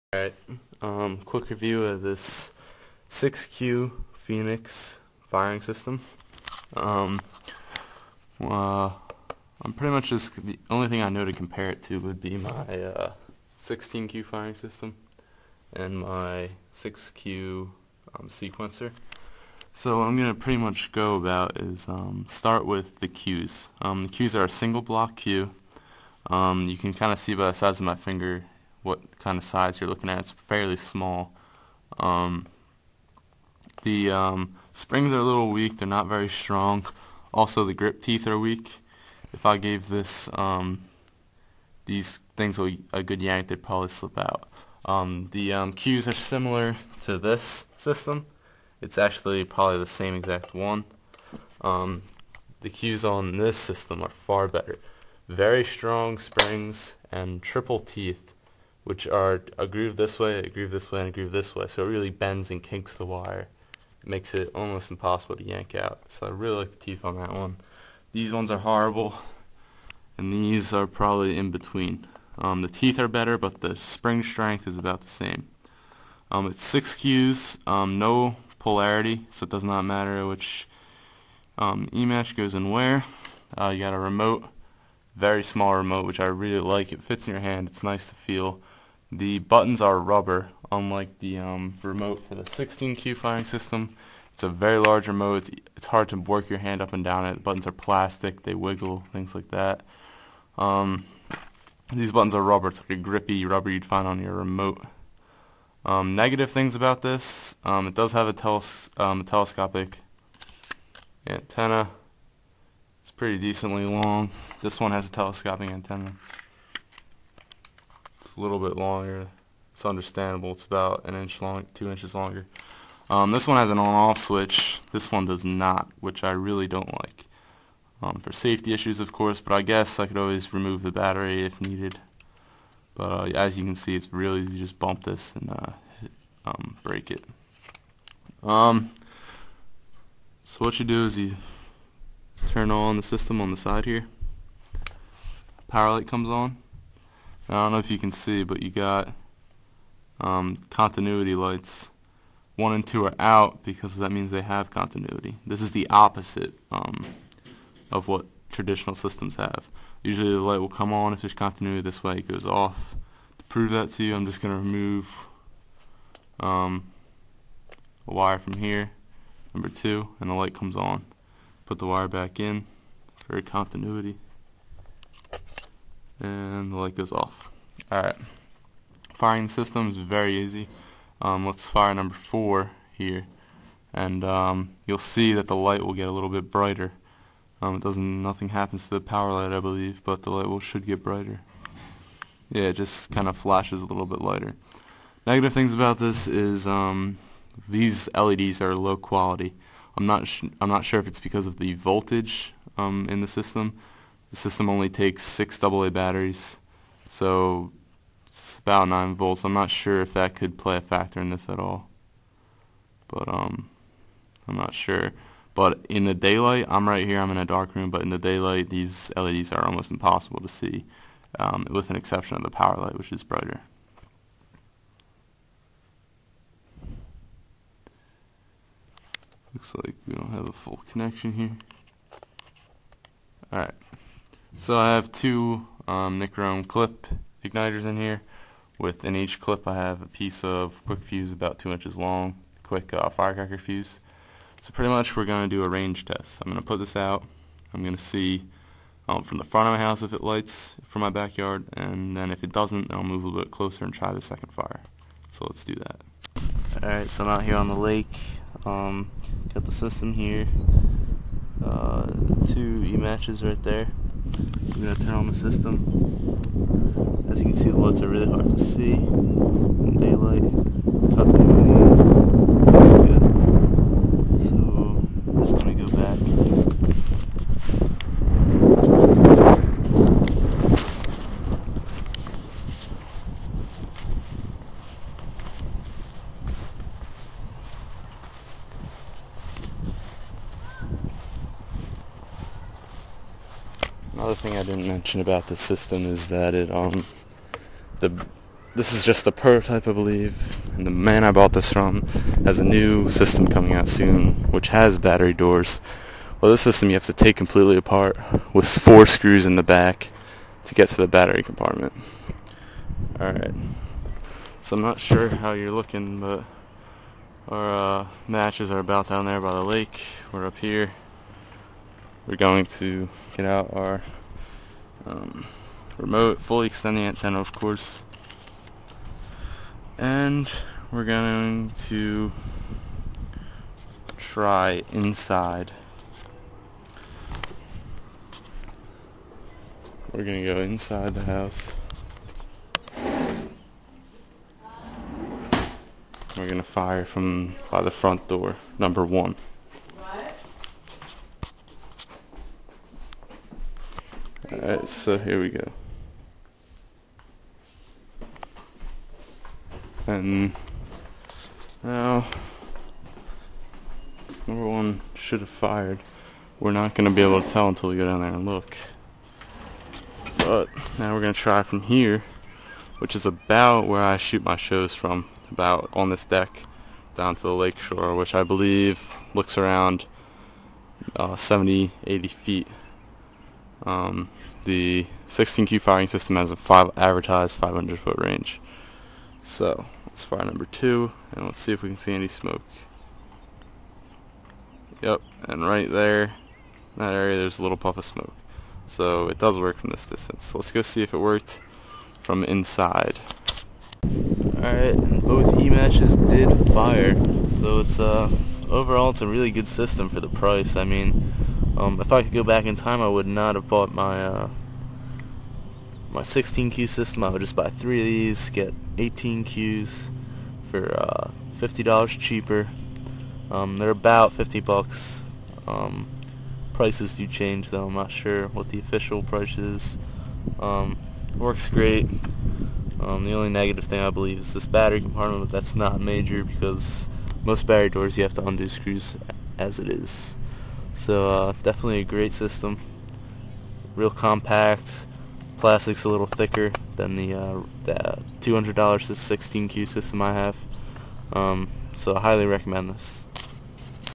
Wireless Firing System Review, 6 cue
6 cue system review and range test

Ken Binkley
Uncle Sam's Fireworks City, Inc.
9805 E, 161st St .
Bixby , OK 74008
USA